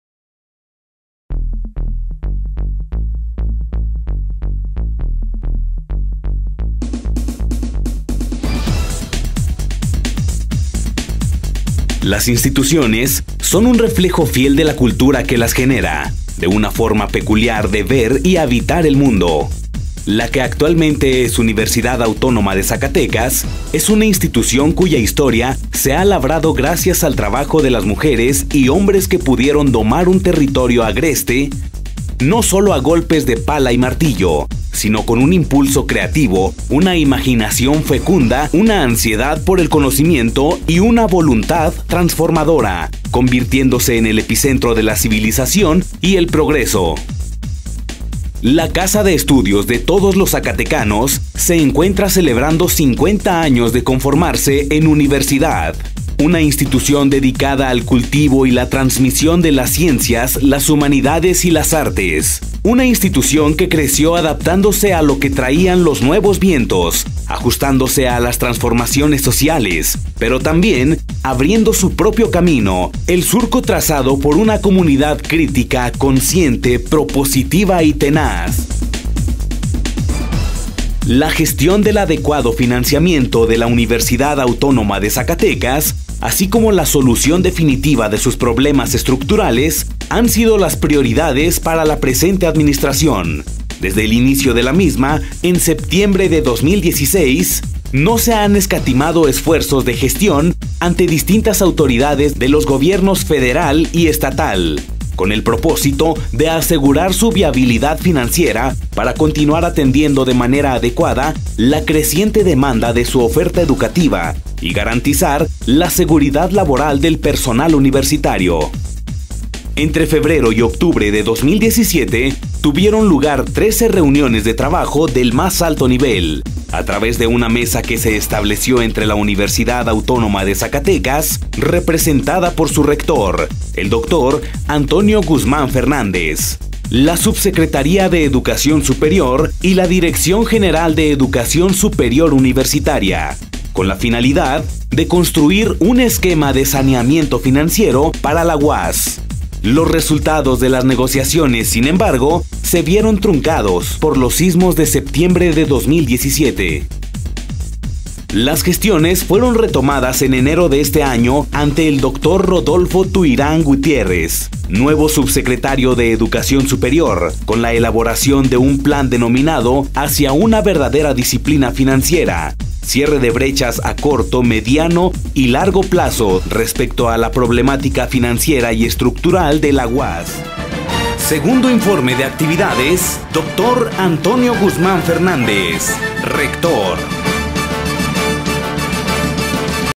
2º Informe Antonio Guzmán Fernández Gestión, Administración y Finanzas
Segundo Informe de actividades del rector Antonio Guzmán Fernández. Gestión, Administración y Finanzas.